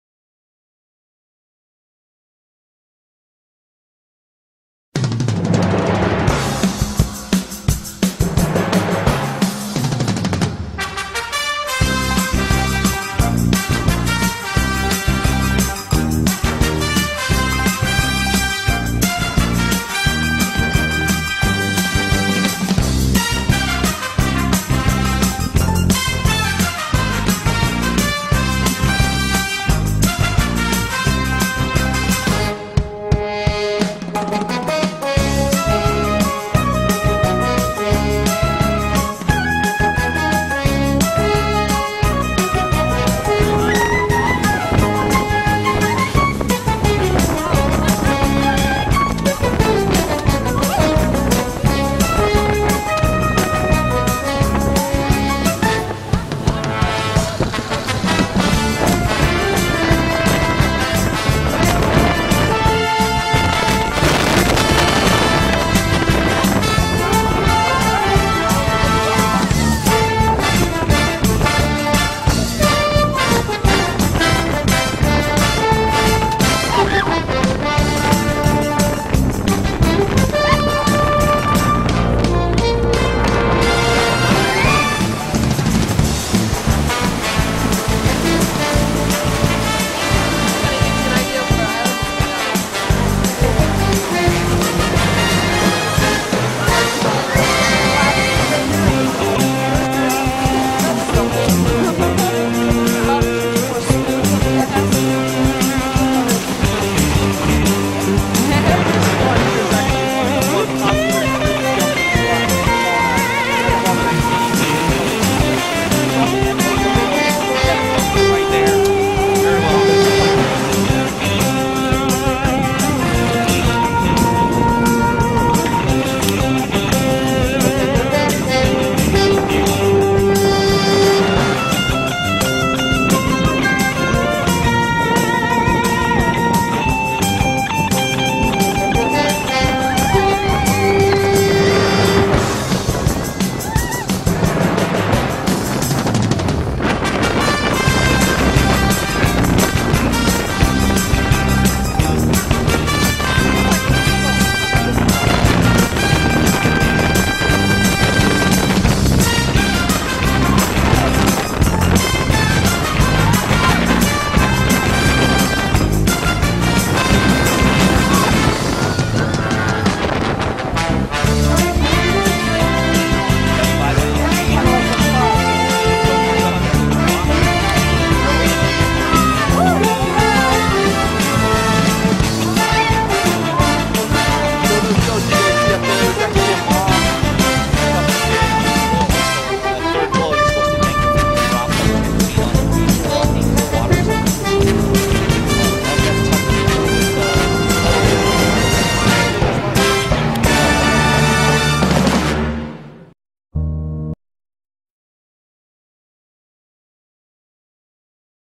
snorkeling.mpg
On our first full day in Kona, Big Island of Hawai'i, we went on a snorkeling outing. Had a blast, saw some awesome fish and coral, and had a rocking boat ride to and from. And this is only the beginning.